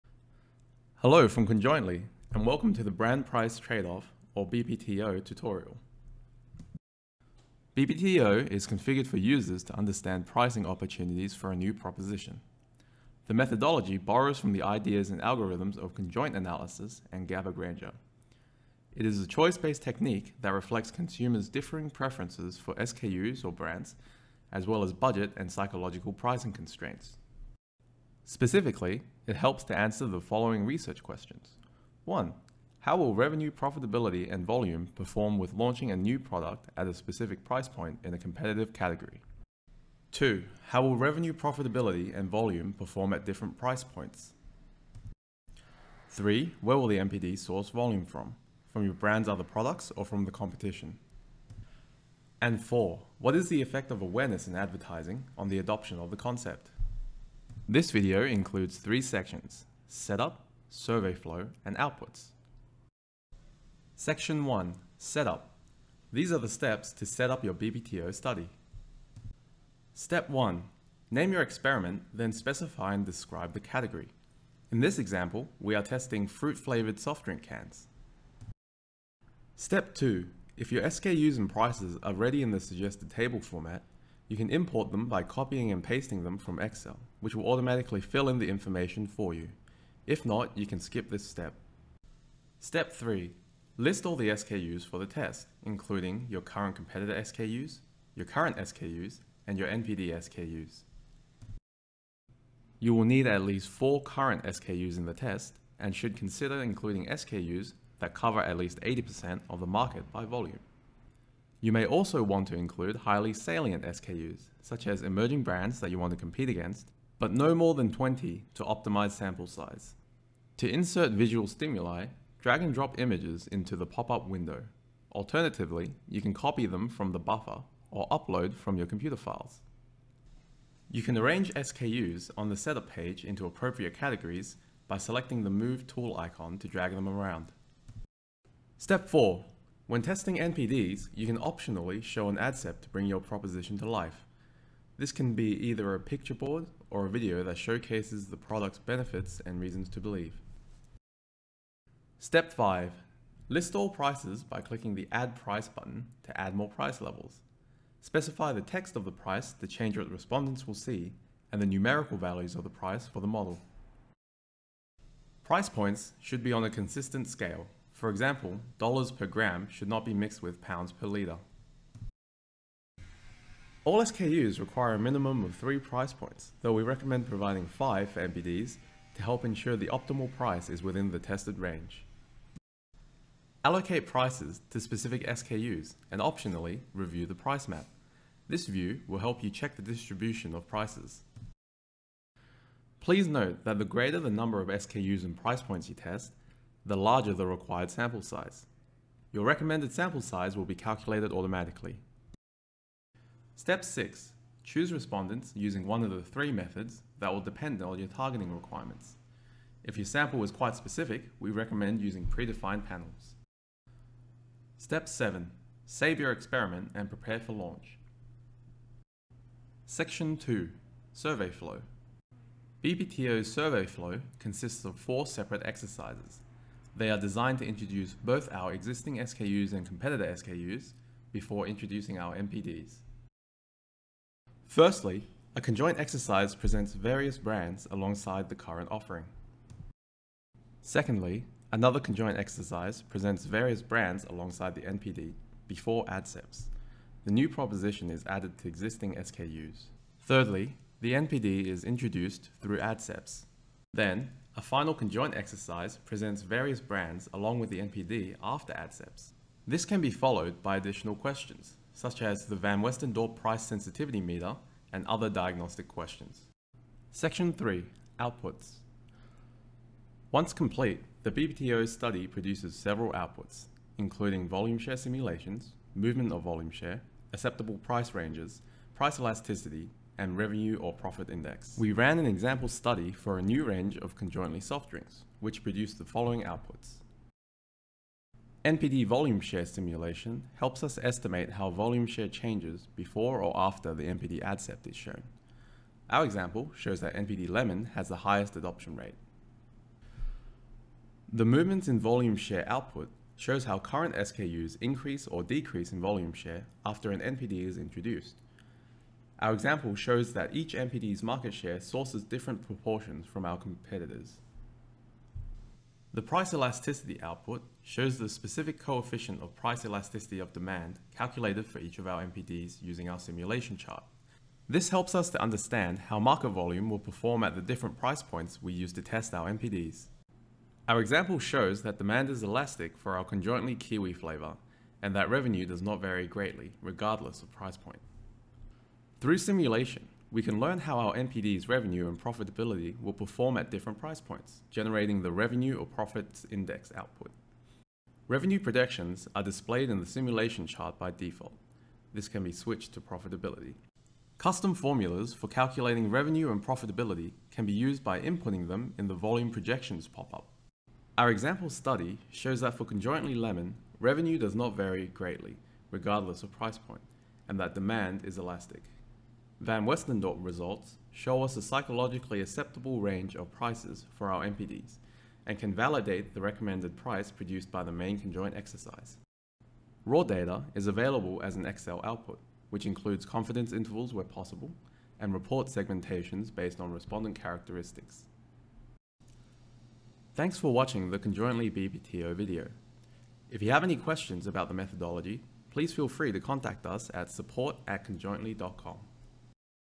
Conjoint.ly - Brand Price Trade Off
Brand-Price Trade-Off (BPTO) is a specialised tool that helps answer pricing questions for consumer goods in a competitive context, such as:

How will revenue, profitability, and market volume perform after launching a new product at a specific price point?
How will revenue, profitability, and market volume perform after re-pricing an existing product?
Where will the NPD source volume from (cannibalise your other products or take share from competition)?
What is the effect of awareness and advertising on the adoption of new concepts?

The methodology borrows from the ideas and algorithms of conjoint analysis and Gabor-Granger. It is a choice-based technique that reflects consumers' differing preferences for SKUs/brands as well as budget and psychological pricing constraints.